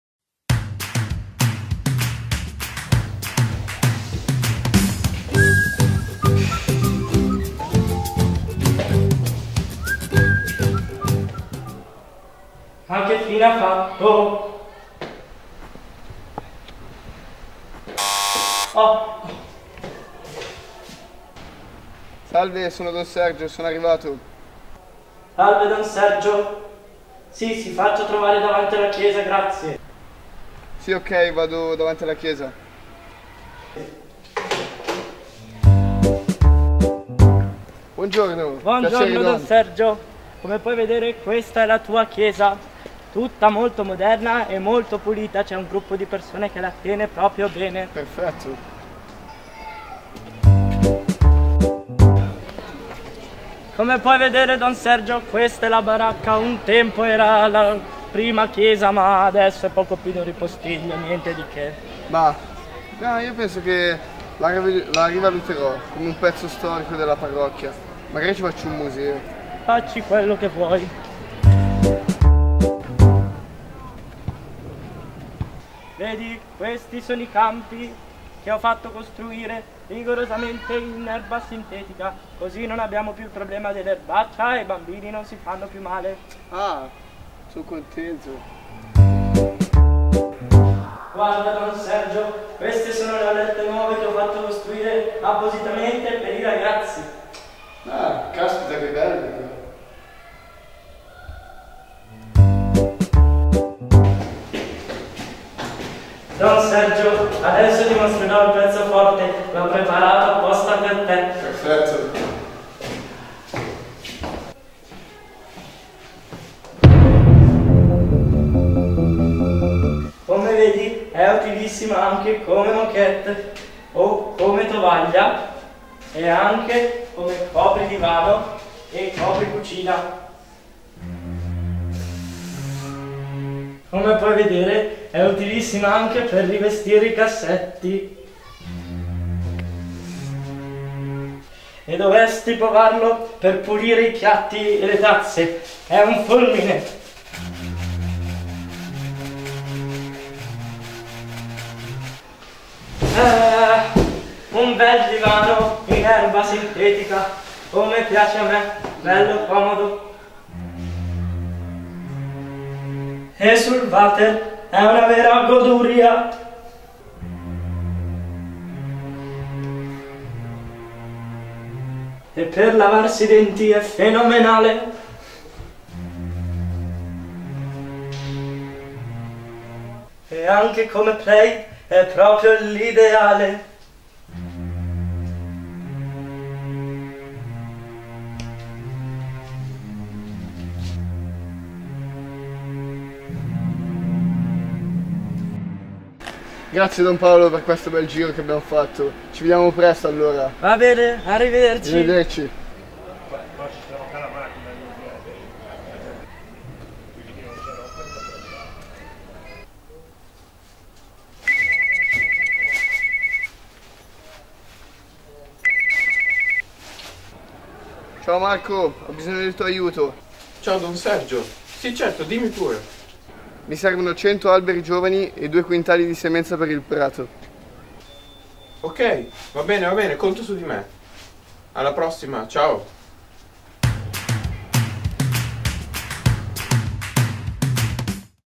PASSAGGIO DI CONSEGNE - Festa di Saluto a Don Paolo
Video ironico proiettato in occasione della festa di saluto a Don Paolo Stefanazzi.

Nei nove anni di servizio presto la nostra Parrocchia, Don Paolo ha fatto molto per la comunità e per gli ambienti parrocchiali.

Ora, però, è giunto il momento di passare tutto nelle mani del nuovo prete, Don Sergio.

In questo video si racconta come sia avvenuto "realmente" il passaggio di consegne.